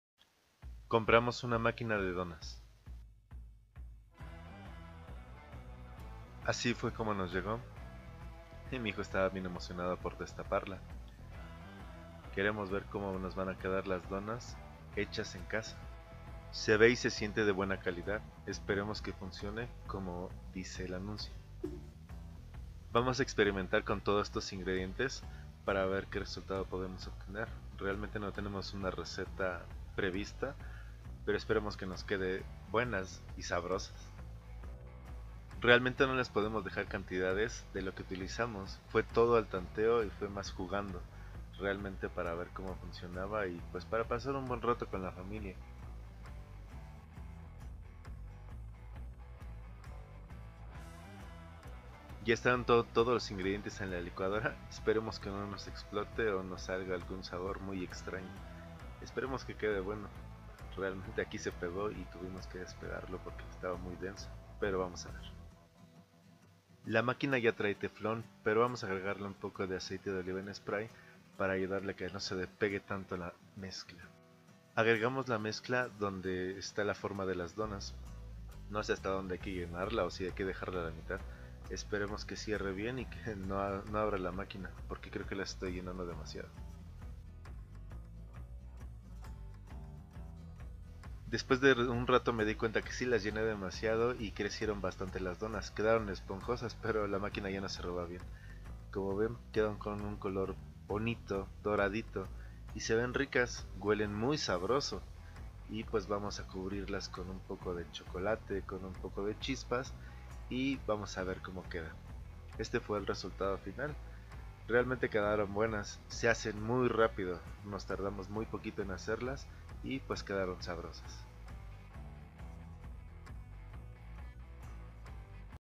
Maquina de donas
La compramos en oferta en bodega Aurrera, se buena, hay que ver que tal funciona